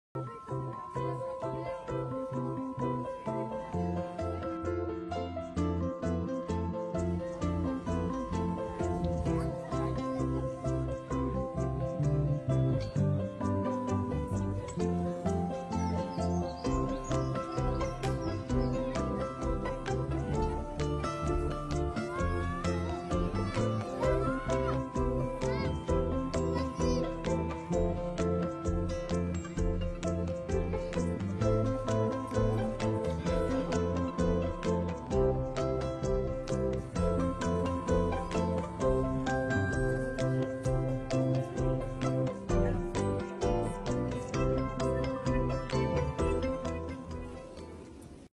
Fête du Printemps 2023
Deuxième édition de la Fête du printemps, qui a reçu le statut de manifestation associée "Fête de la Nature 2023".